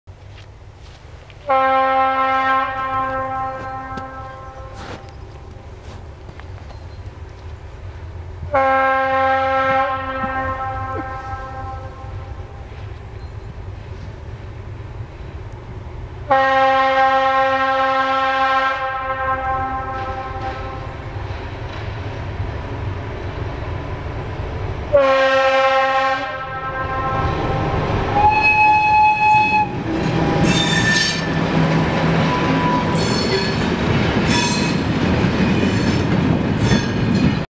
742 209 pozdrav + zaměstnanec ČD
Natočeno 15.února 2017 ve 12:35 ve Vlastějovicích.